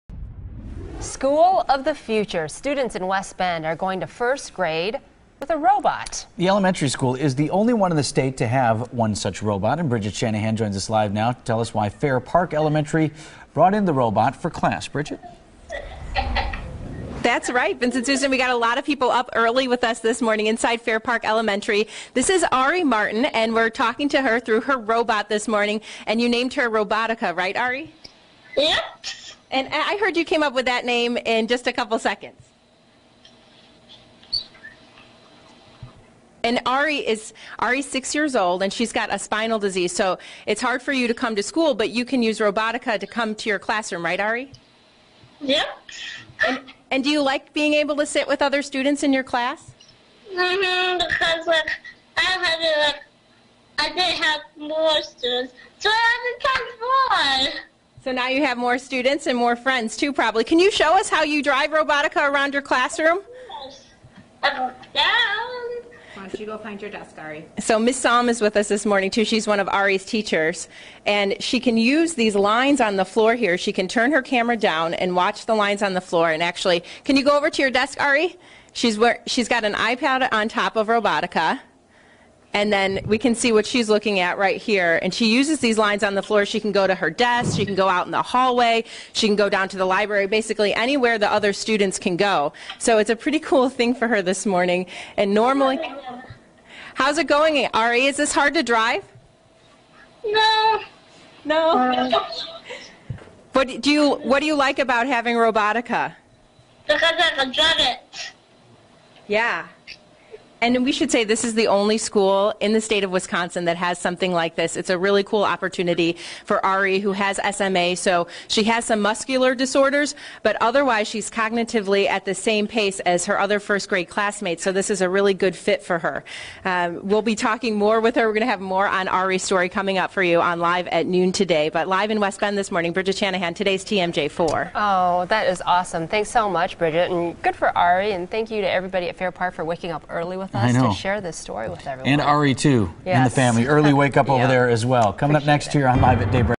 West Bend elementary school introduces robot to classroom
Bridget Shanahan's 6:30 a.m. report